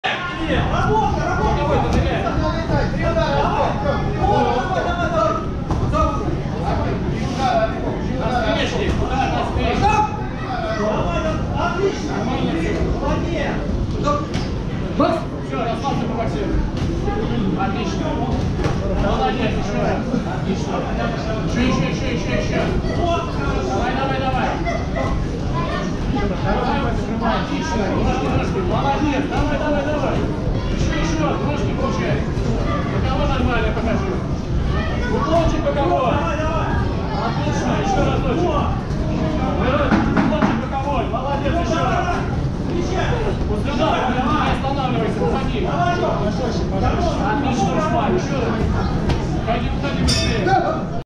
Олйникова Александра Туровцева Екатерина5
5 октября 2019 года. Турнир 7 легенд (1 турнир) - Кубок Бориса Лагутина. Зал на Кожуховской.